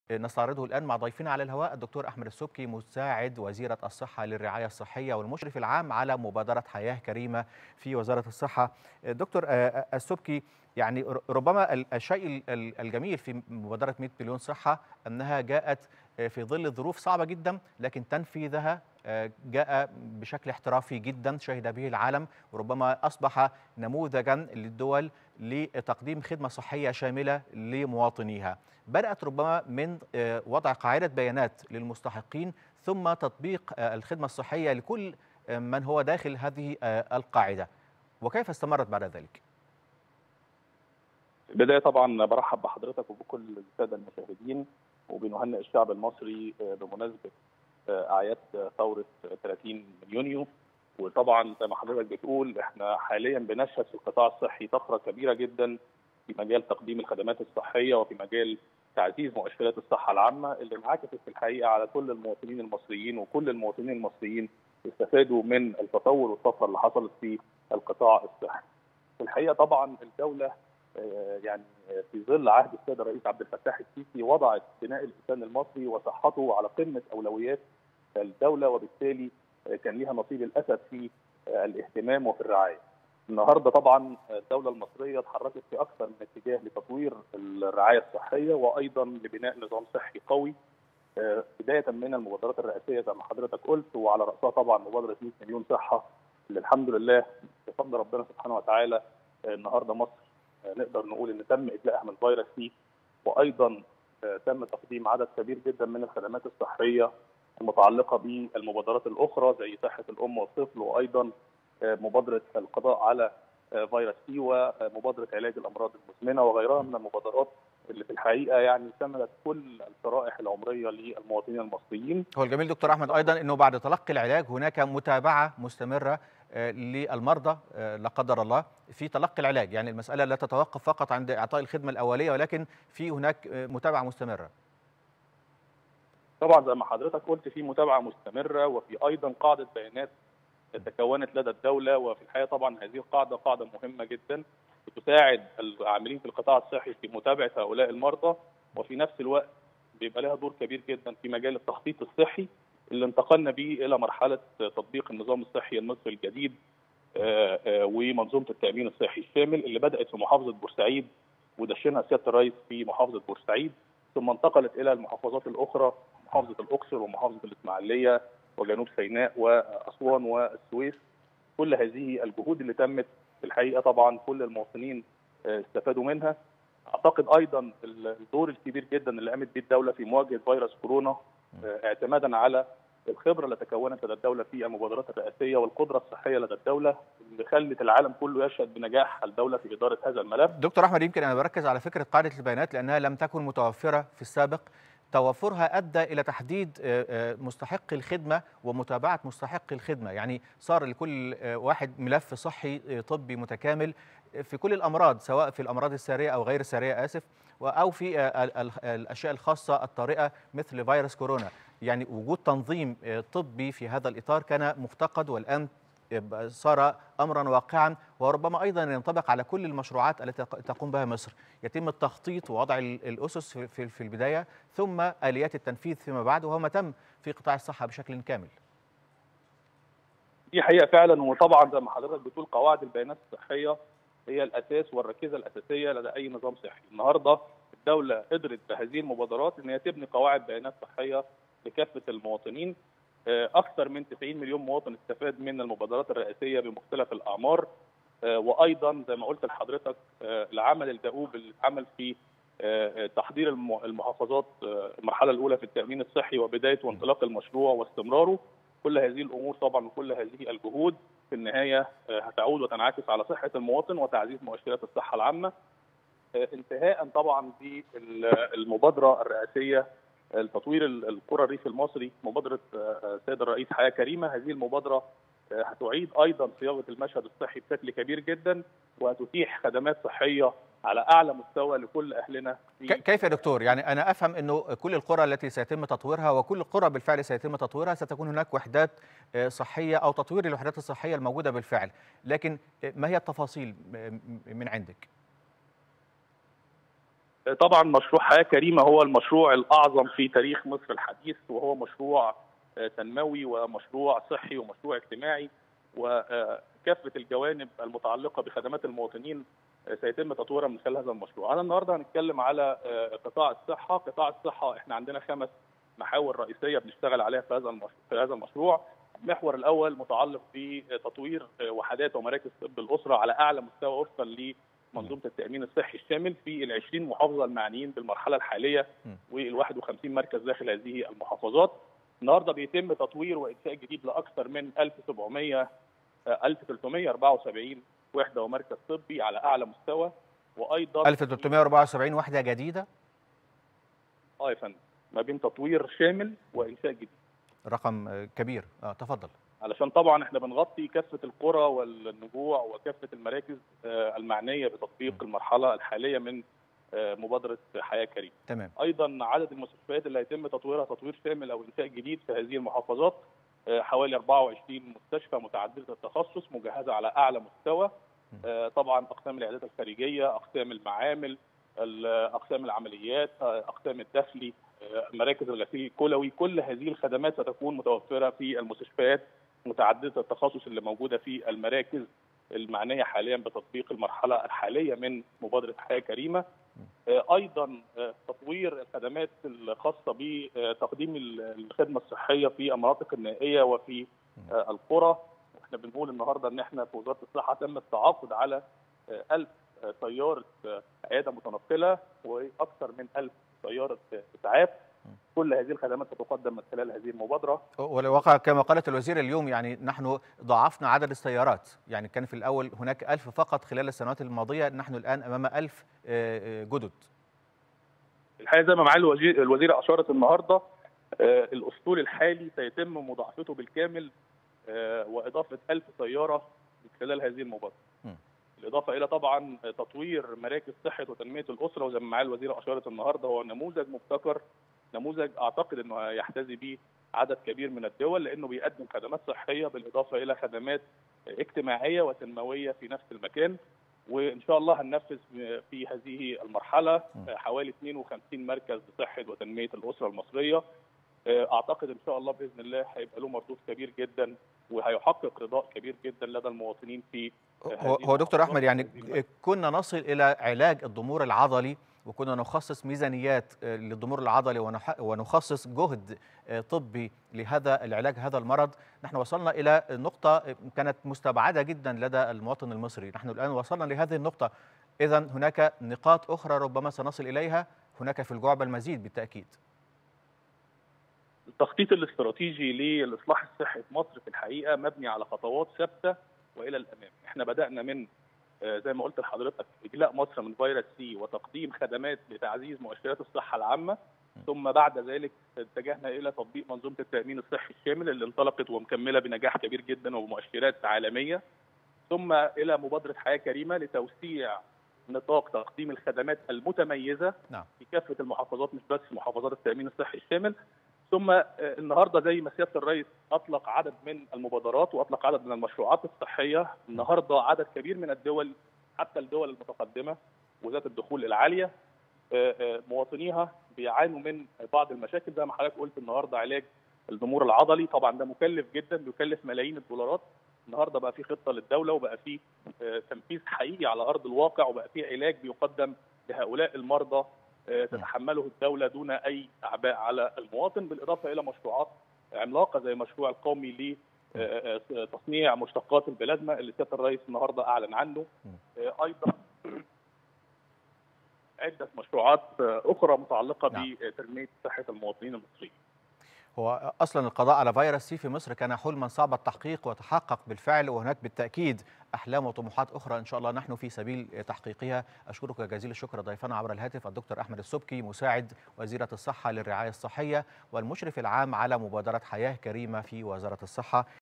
30 يونيو | مساعد وزيرة الصحة يوضح إنجازات الدولة في تحسين المنظومة الصحية
30 يونيو | مساعد وزيرة الصحة يوضح إنجازات الدولة في تحسين المنظومة الصحية

30  يونيو 2021